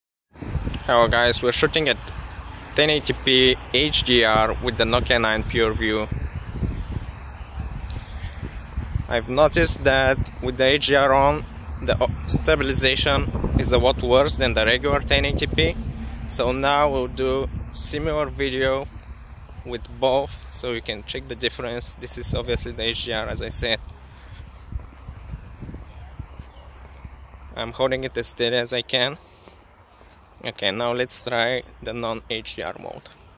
Nokia 9 PureView 1080p HDR Sample Video 1
This is a sample video taken with the Nokia 9 PureView.